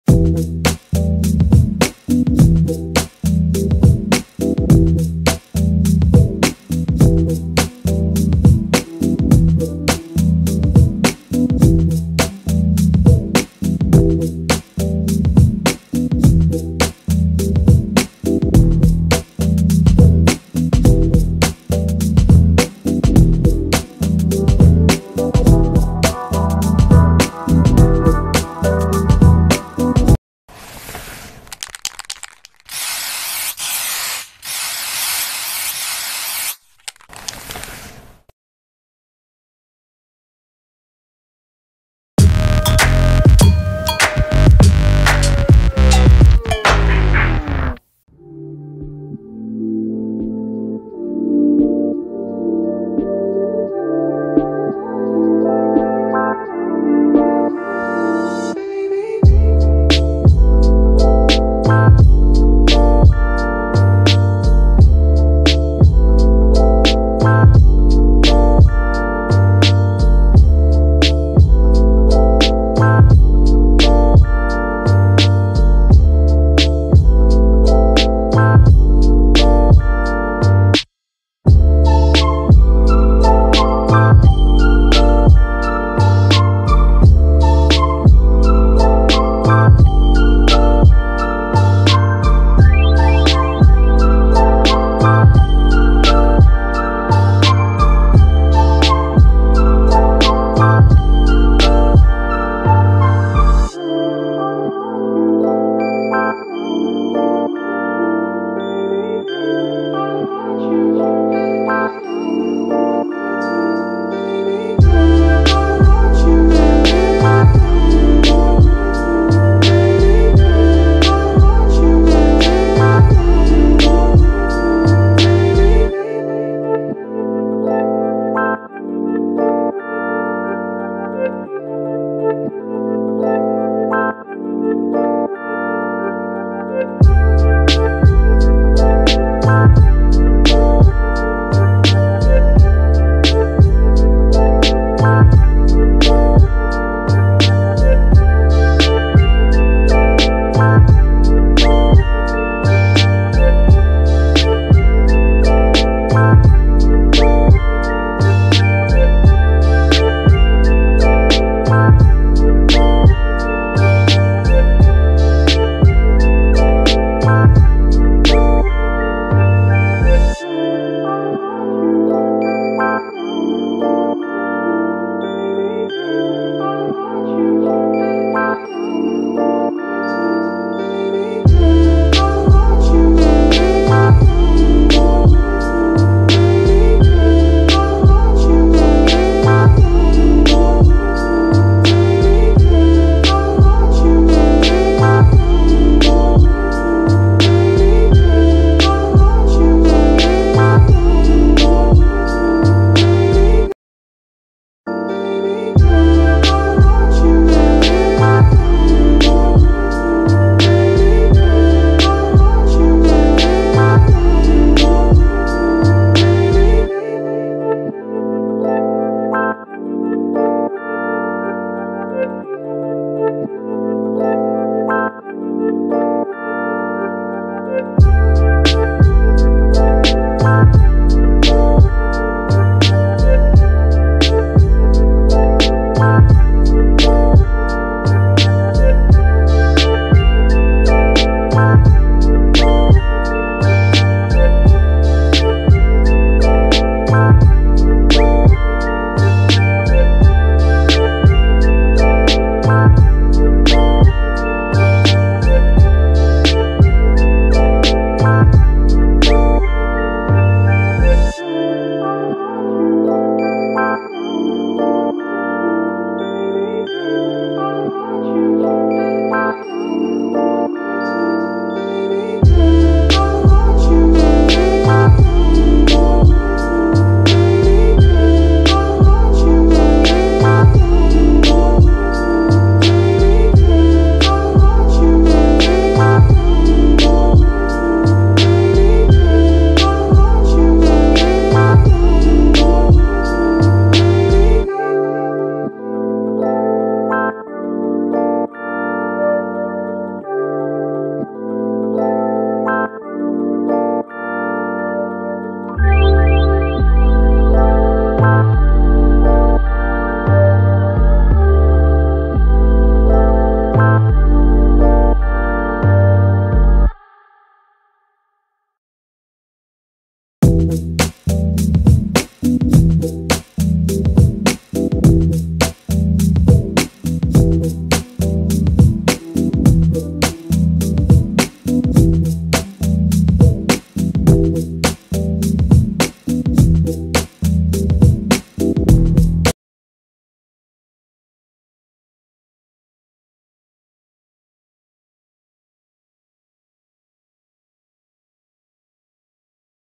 UNBOXING NIKE ISPA OVERREACT "LIGHT BONE" & RAFFLE!
I will be unboxing the new NIKE ISPA Overreact "Light Bone" was release on July 09 2020.